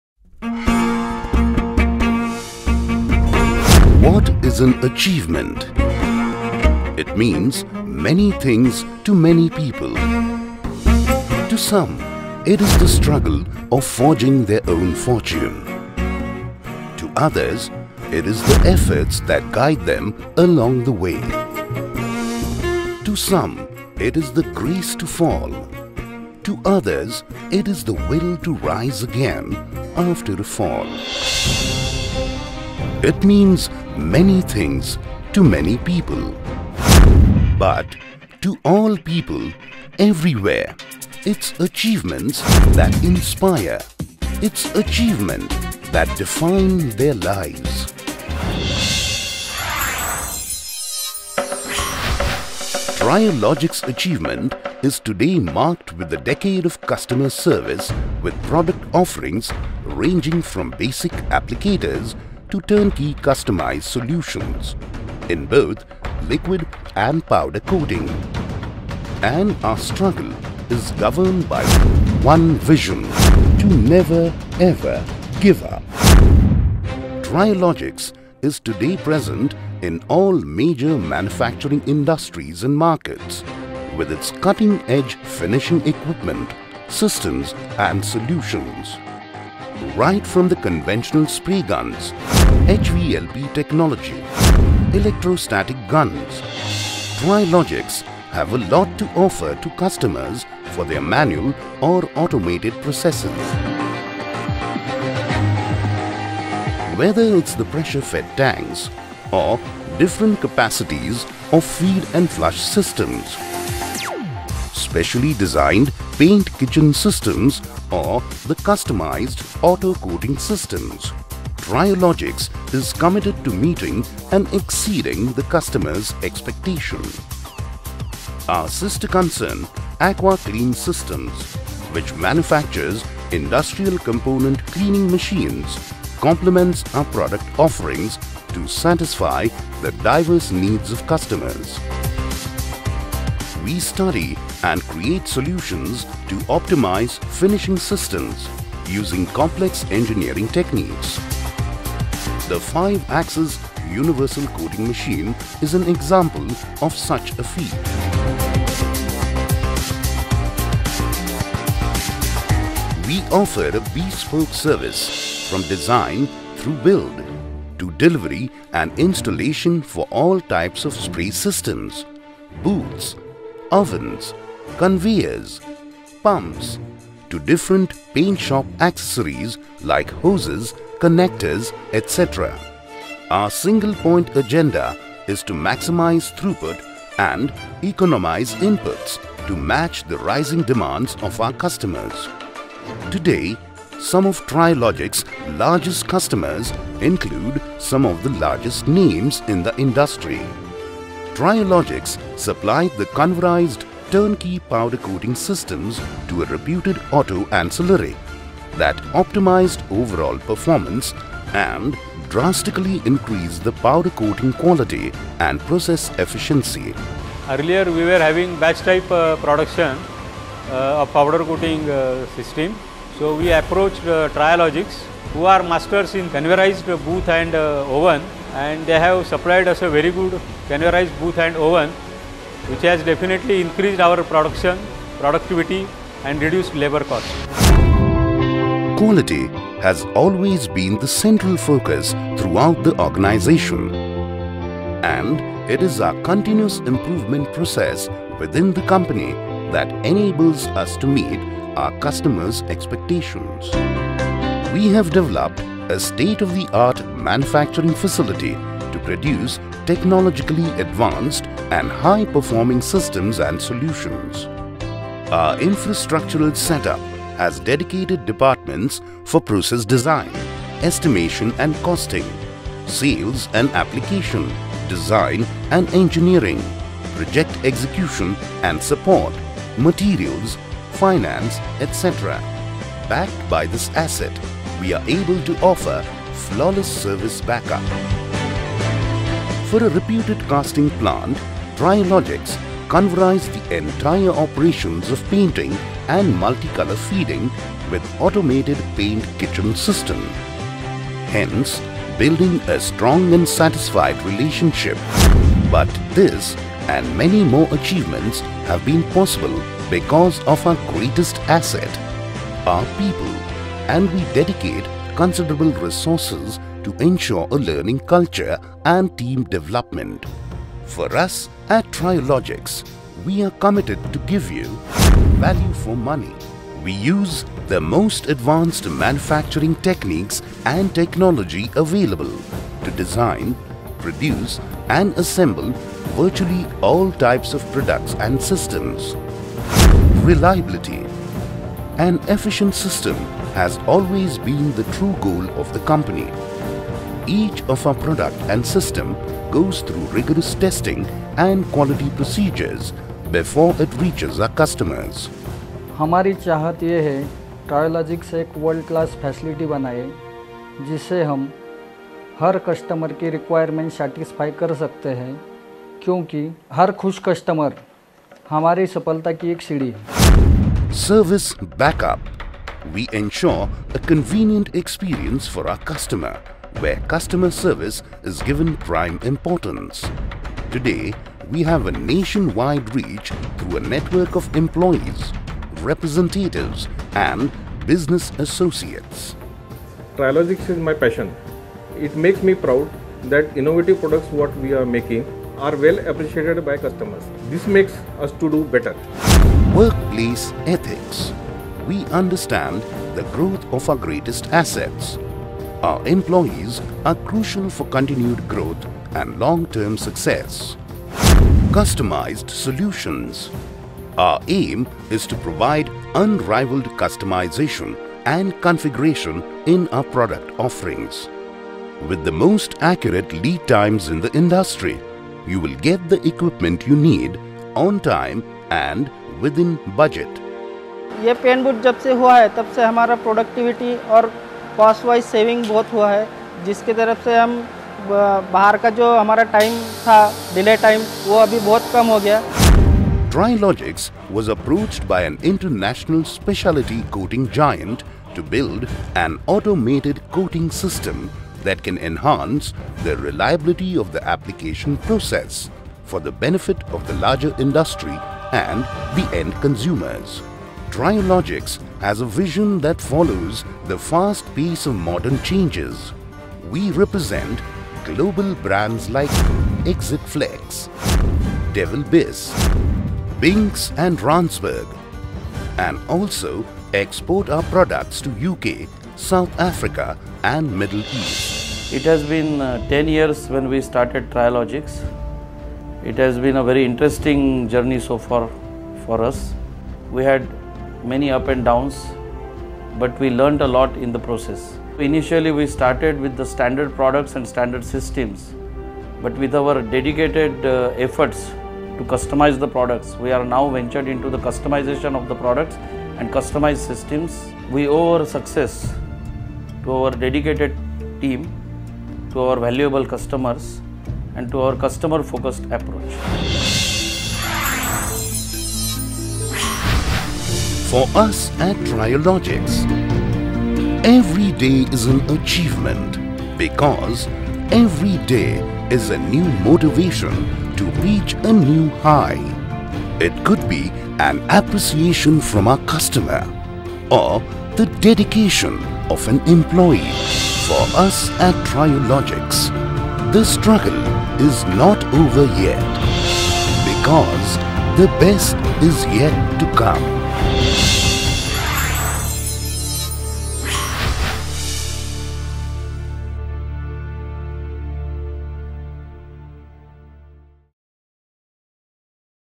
Triologics (Bullows) Old Corporate Video - 2012
Triologics founded in 2002, mainly serving market with wide range of Spray Painting Equipment, Dispensing Systems under Bullows Paint Equipment Private Limited, India. In 2012, Triologics completed it's 10 years of market existence. Corporate video is to show Infrastructure & Customer Testimonials.